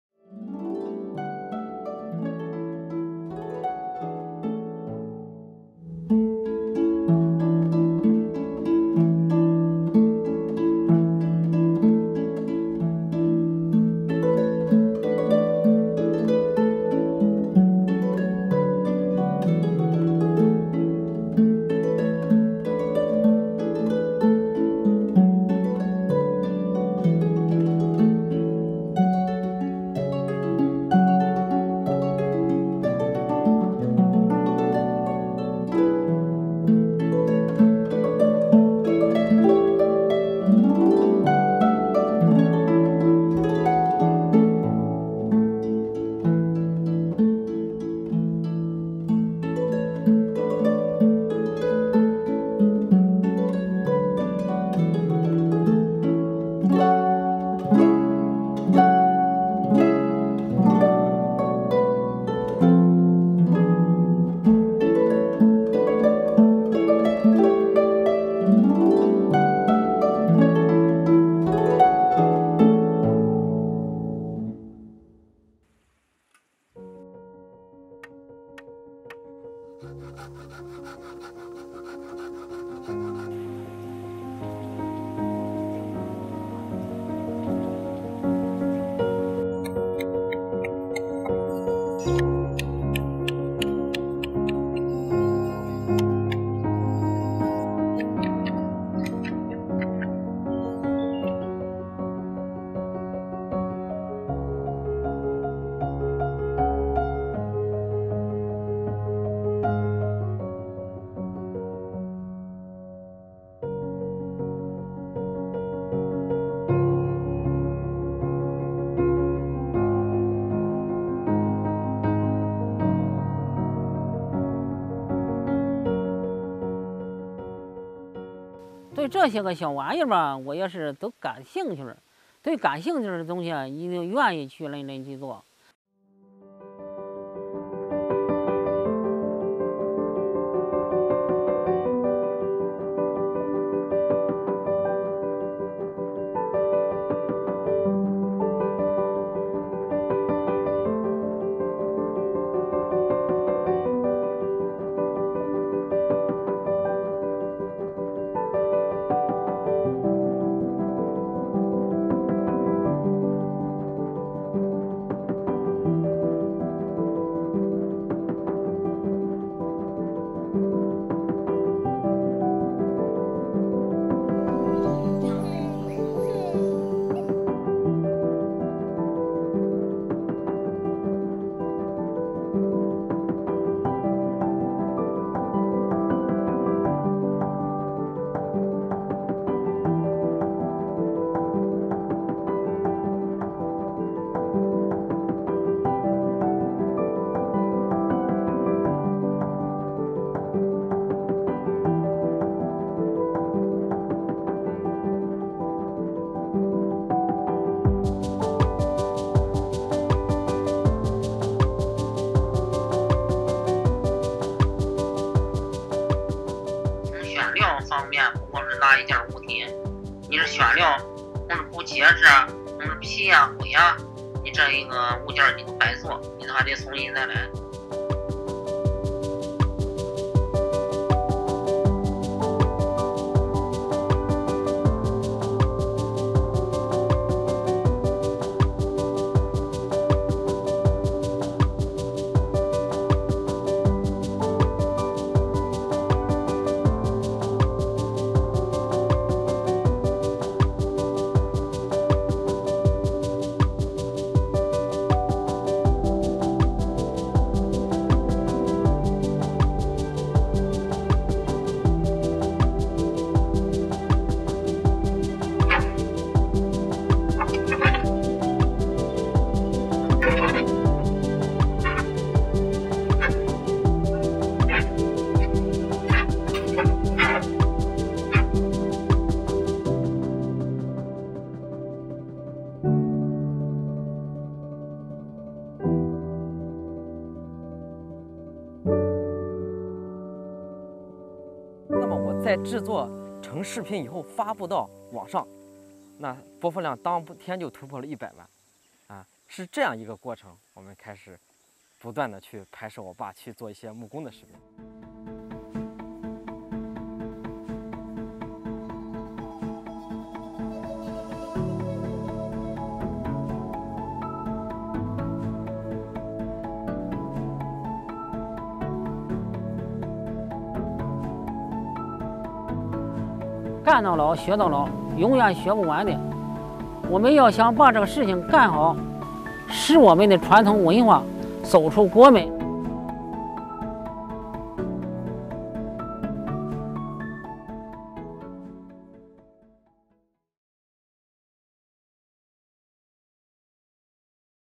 Chinese master carpenter Grandpa Amu creates intricate toys for his grandson
“Grandpa Amu”, from eastern China’s Shandong province, is a master carpenter with millions of fans on social media around the world. He uses an ancient woodworking technique to create intricate toys for his young grandson. He shares his videos on Xigua Video, a Chinese video platform, on which he has attracted millions of followers.